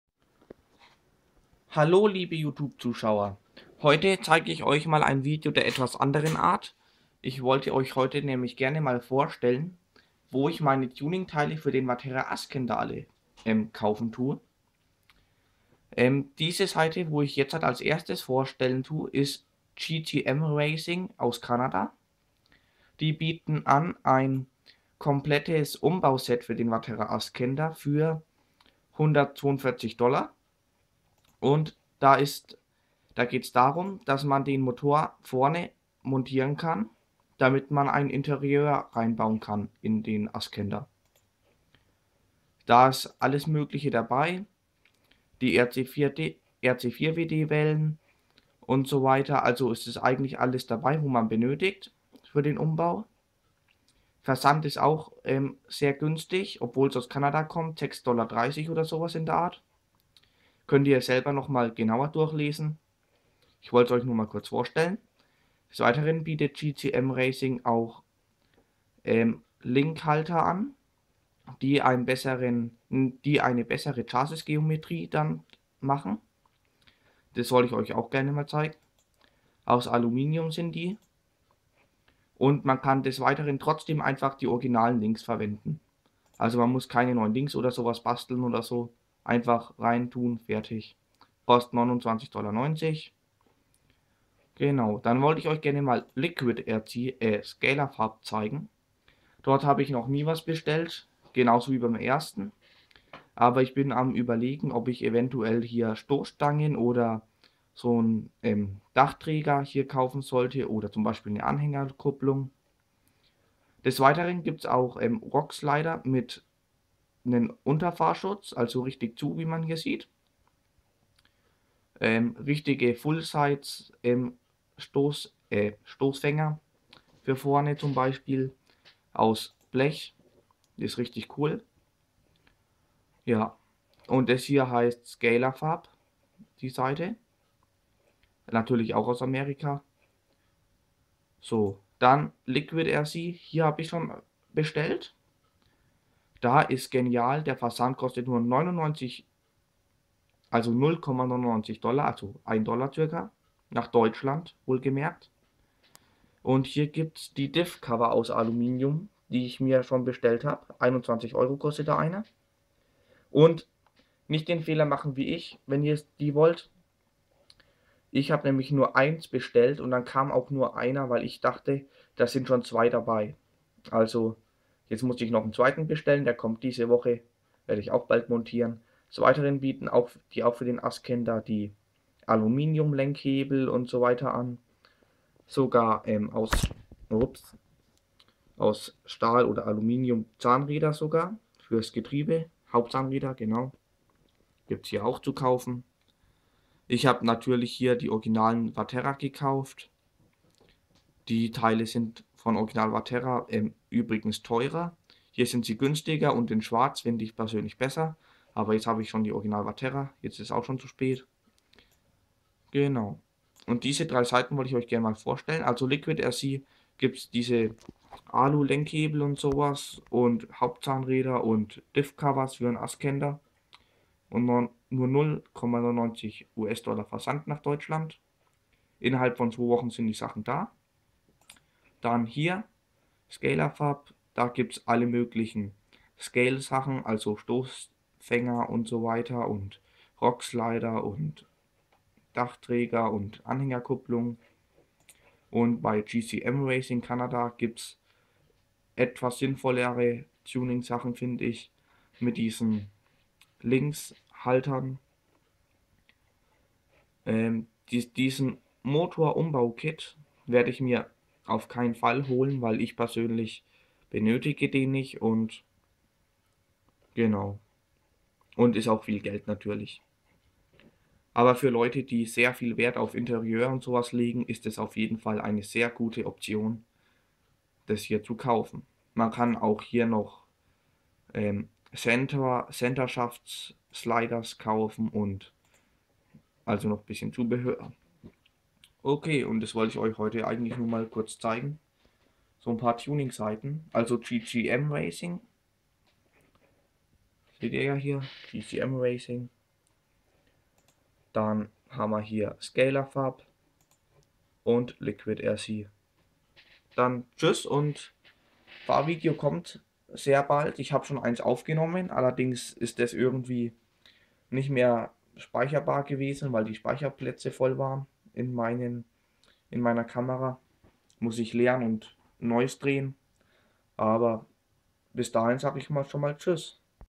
Tuningseiten für den Vaterra Ascender
In Diesem Video stelle ich euch Seiten vor bei denen ihr gutes Tuning für den Ascender bekommt.